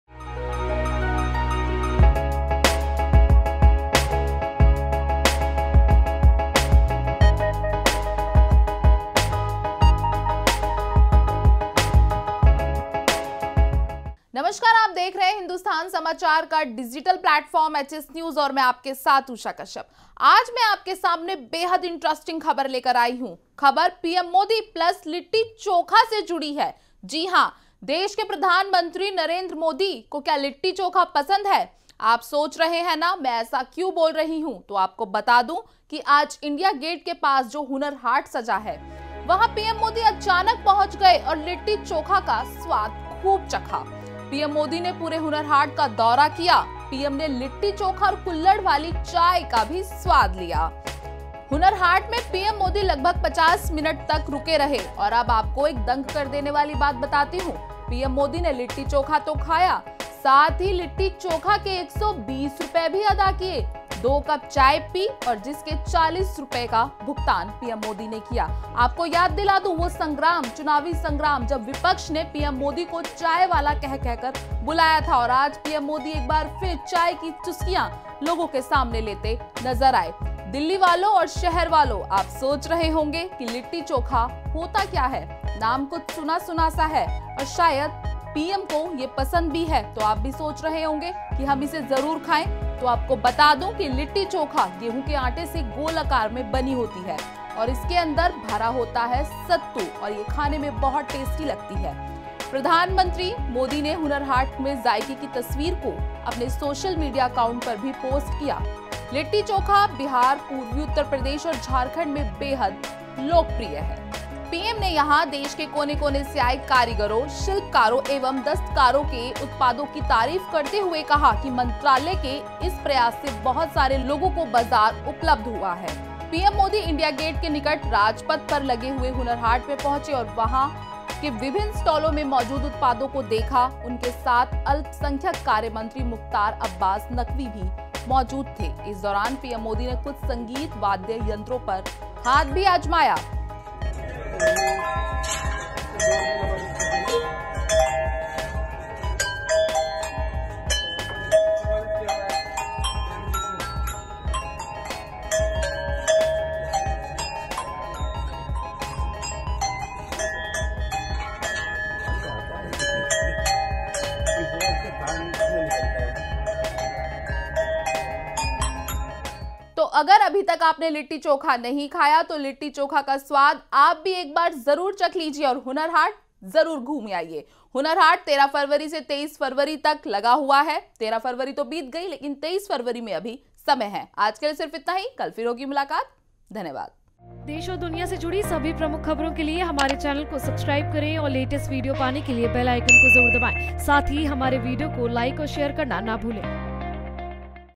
PM Modi को भाया बिहारी व्यंजन, Hunar Haat में खाया लिट्टी चोखा
प्रधानमंत्री नरेंद्र मोदी ने अल्पसंख्यक कार्य मंत्रालय की ओर से आयोजित ‘हुनर हाट’ का आज  को दौरा किया और देश के कोने-कोने से आए कारीगरों, शिल्पकारों एवं दस्तकारों के उत्पादों की तारीफ करते हुए कहा कि मंत्रालय के इस प्रयास से बहुत सारे लोगों को बाजार उपलब्ध हुआ है।